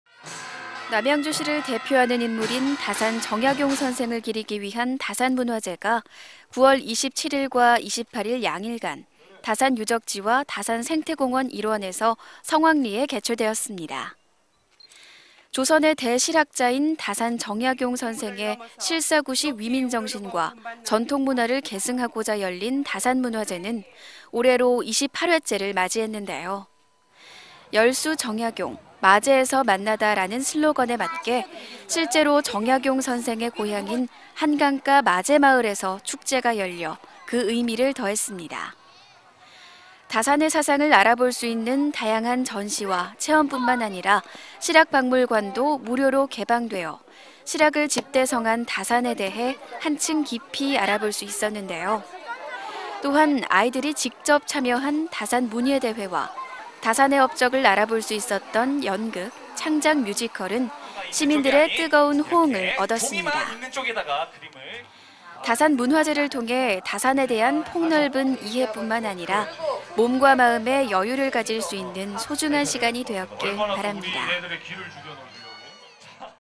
남양주시 쾌TV - 제 28회 다산문화제 개최
남양주시를 대표하는 인물인 다산 정약용선생을 기리기 위한 다산문화제가 9월 27일과 28일 양일간 다산유적지와 다산생태공원 일원에서 성황리에 개최되었습니다.

조선의 대 실학자인 다산 정약용선생의 실사구시 위민정신과 전통문화를 계승 하고자 열린 다산문화제는 올해로 28회째를 맞이했는데요. ‘열수(洌水) 정약용! 마재에서 만나다’라는 슬로건에 맞게 실제로 정약용선생의 고향인 한강가 마재마을에서 축제가 열려 그 의미를 더했습니다.

다산의 사상을 알아볼 수 있는 다양한 전시와 체험뿐만 아니라 실학박물관도 무료로 개방되어 실학을 집대성한 다산에 대해 한층 깊이 알아볼 수 있었는데요. 또한 아이들이 직접 참여한 다산문예대회와 다산의 업적을 알아볼 수 있었던 연극, 창작뮤지컬은 시민들의 뜨거운 호응을 얻었습니다.

다산문화제를 통해 다산에 대한 폭넓은 이해뿐만 아니라 몸과 마음에 여유를 가질 수 있는 소중한 시간이 되었기를 바랍니다.